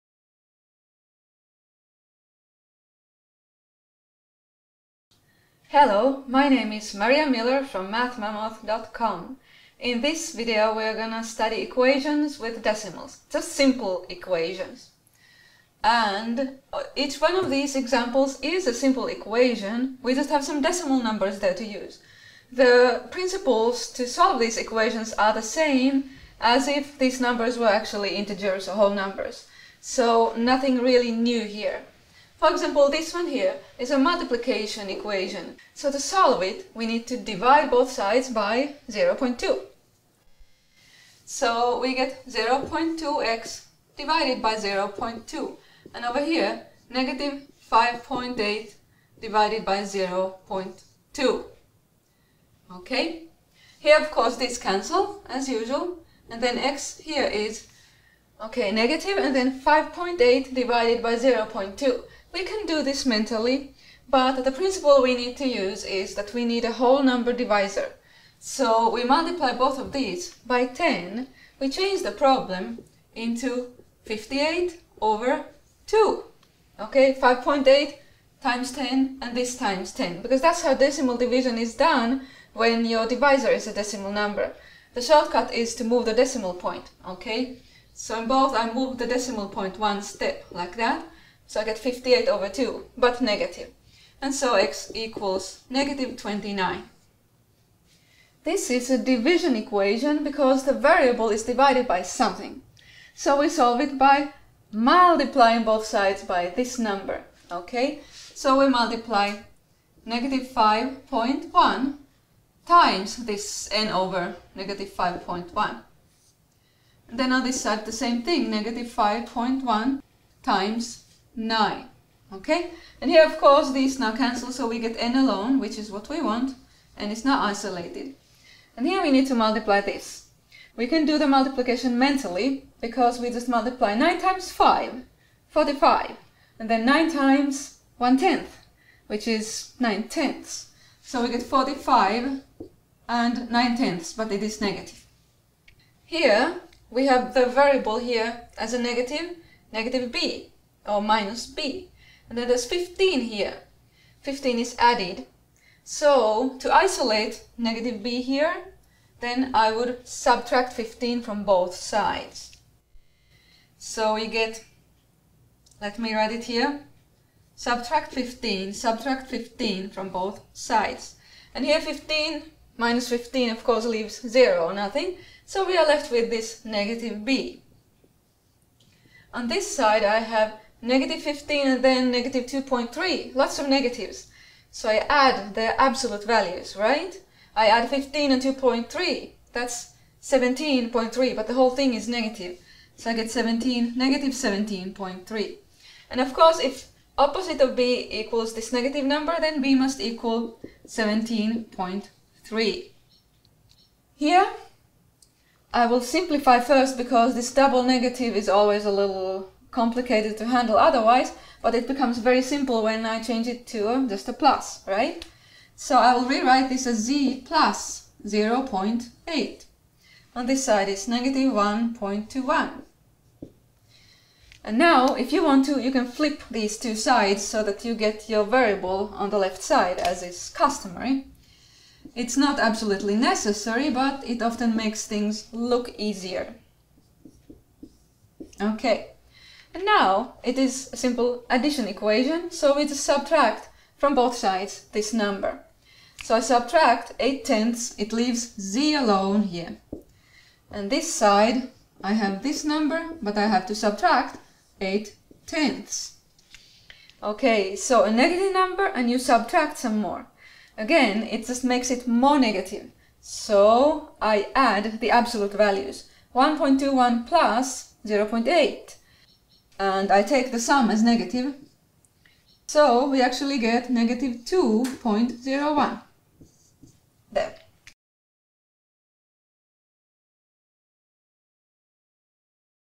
Simple equations with decimals - pre-algebra/7th grade math
A lesson for pre-algebra students: one-step equations that involve positive and negative decimals.  I solve four simple equations as examples, explaining each step of the solution.

The 2nd part of this lesson has to do with equations involving percentages.